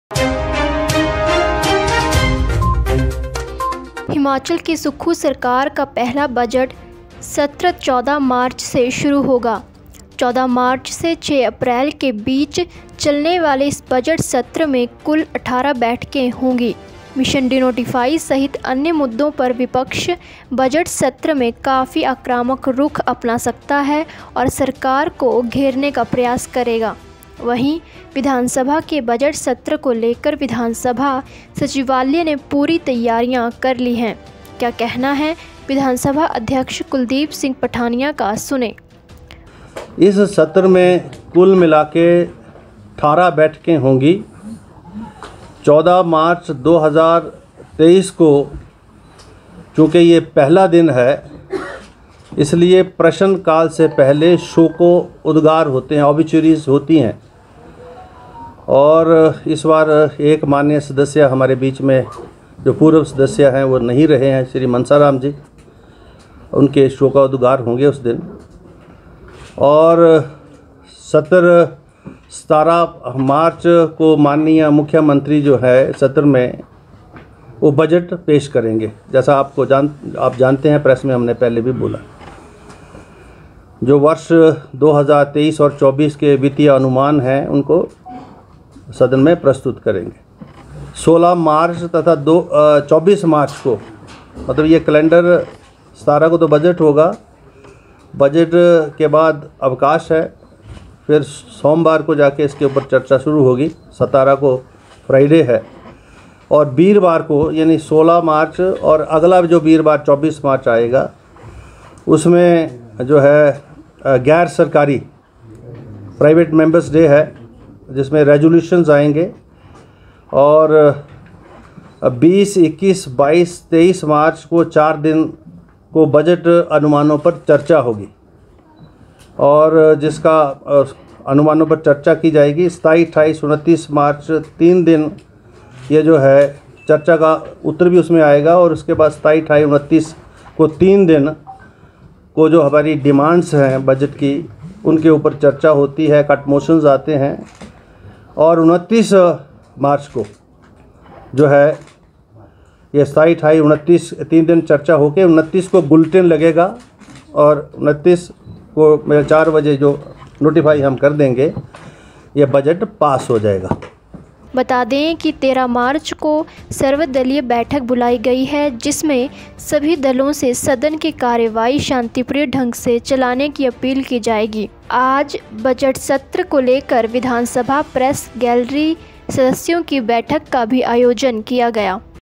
हिमाचल विधानसभा सत्र को तैयारियां पूरी, 17 मार्च को बजट होगा पेश, 29 को पारित | HP Budget Session
The first budget session of Himachal's Sukhu government will begin from March 14. There will be a total of 18 sittings in this budget session, which will run from March 14 to April 6. CM Sukhwinder Singh Sukhu will present the budget in the House on March 17